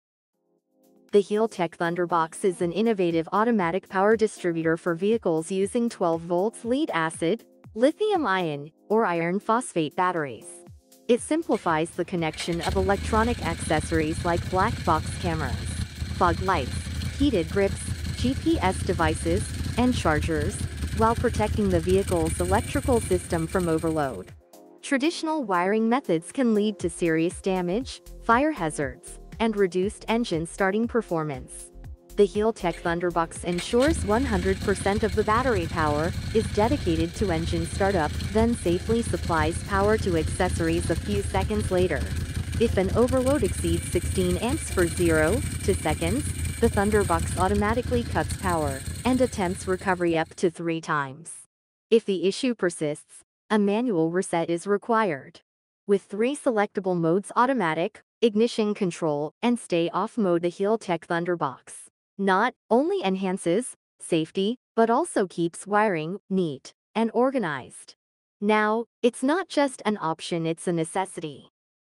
HealTech ThunderBox Test Video (TB U01, TB U02)
The HealTech Thunderbox is an innovative automatic power distributor for vehicles using 12V lead-acid, lithium-ion, or iron phosphate batteries. It simplifies the connection of electronic accessories like black box cameras, fog lights, heated grips, GPS devices, and chargers, while protecting the vehicle’s electrical system from overload.

Traditional wiring methods can lead to serious damage, fire hazards, and reduced engine starting performance. The HealTech Thunderbox ensures 100% of the battery power is dedicated to engine startup, then safely supplies power to accessories a few seconds later.

If an overload exceeds 16 amps for 0.2 seconds, the Thunderbox automatically cuts power and attempts recovery up to three times. If the issue persists, a manual reset is required.

With three selectable modes—automatic, ignition control, and stay-off mode—the HealTech Thunderbox not only enhances safety but also keeps wiring neat and organized.

Now, it's not just an option—it’s a necessity.